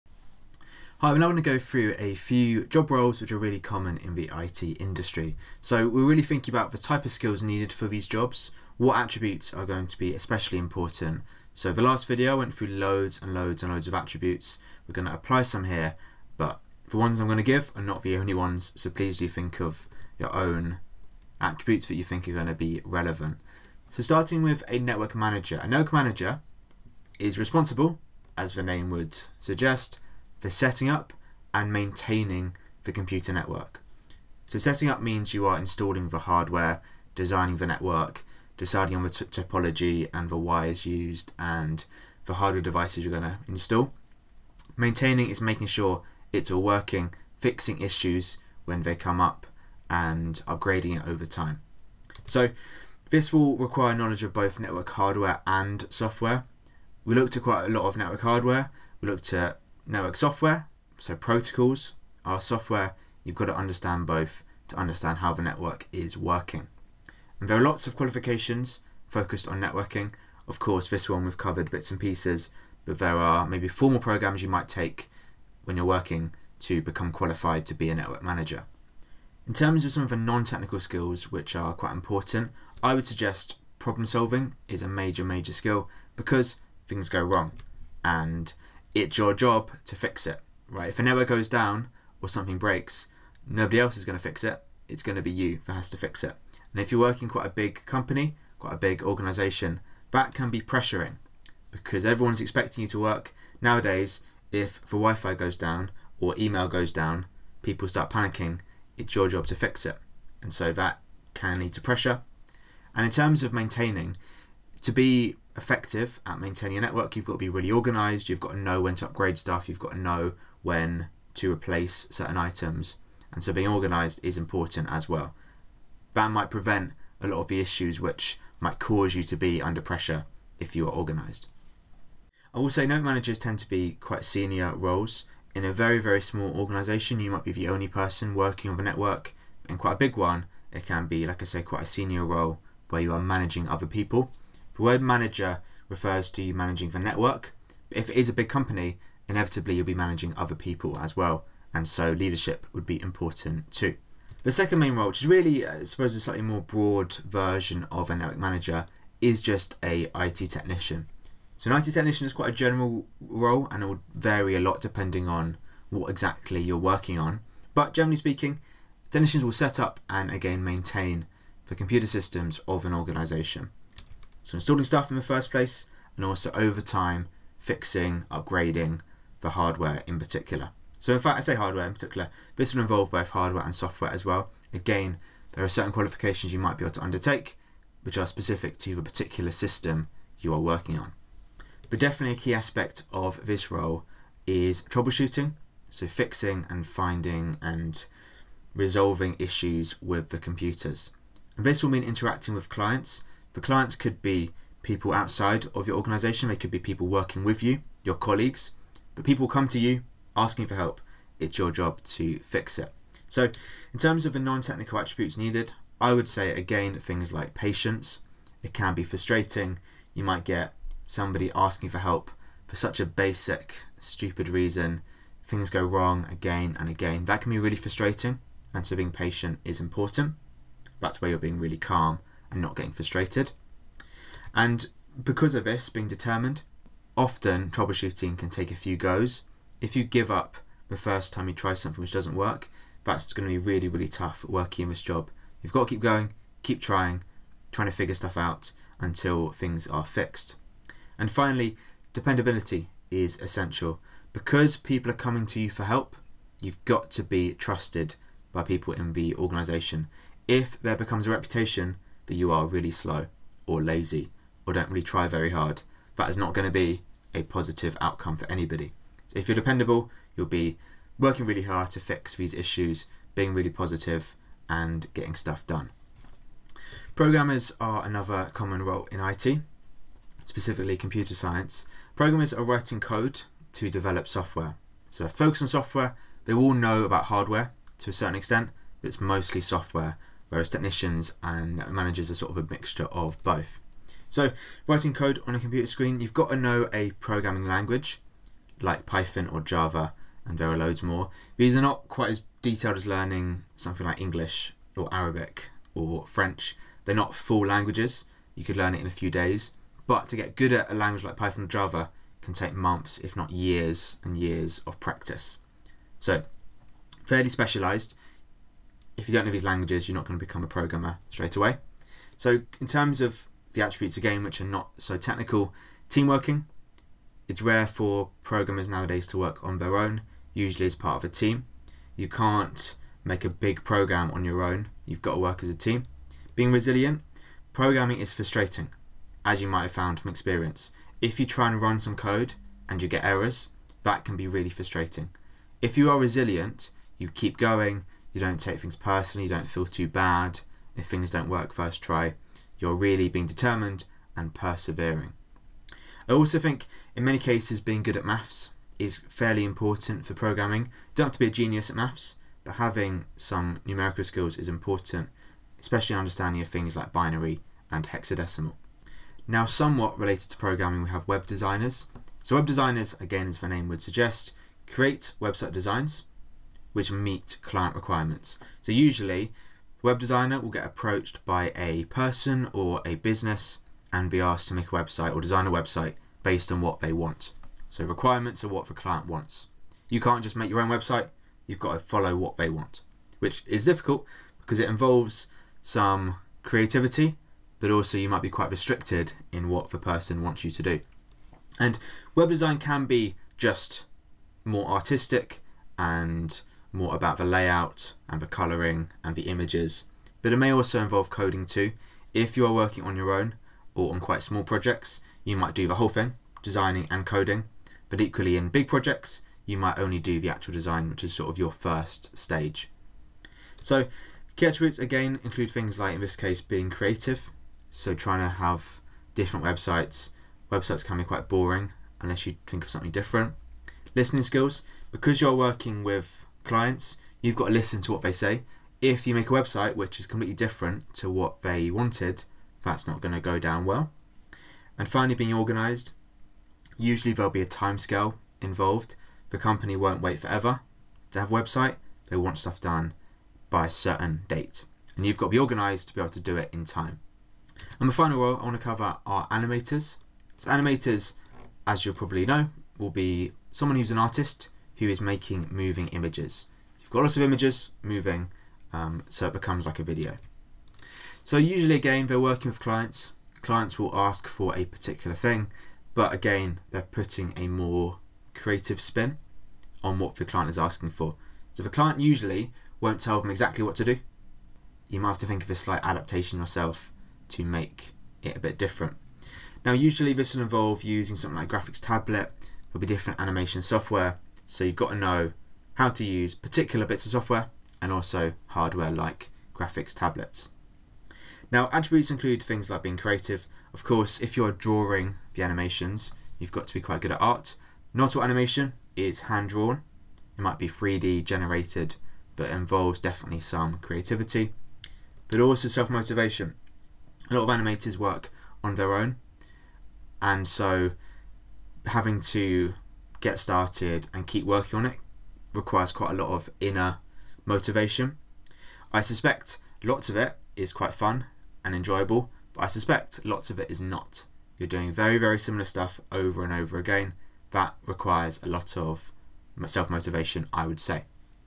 Common Job Roles in IT
Outlining the key requirements and attributes of network managers, IT technicians, programmers, web designers, and animators.

This video is primarily designed for the written Unit 1 exam (Fundamentals of IT) for the OCR Cambridge Technicals (CTEC) Level 3 Information Technology qualifications. Video #4 of LO4 (Understand employability and communication skills used in an IT environment).

For the ordered playlist of videos for the CTEC IT Fundamentals of IT (Unit 1) exam,